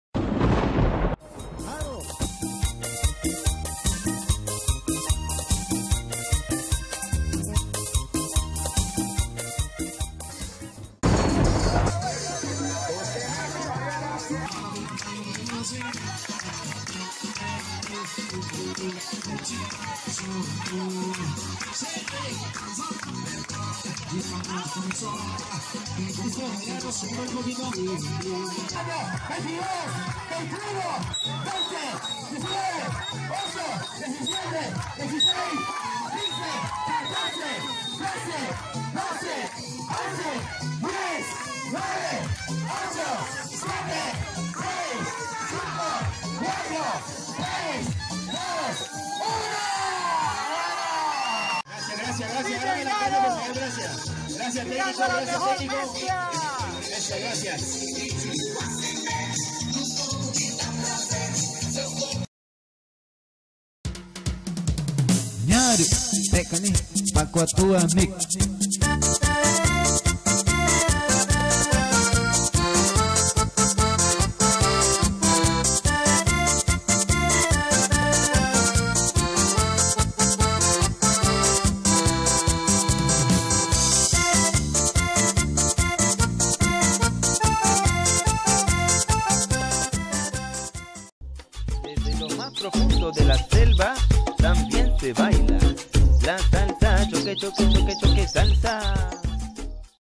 Maraton bailable en Leticia
8 horas de baile música de la triple frontera en el barrio porvenir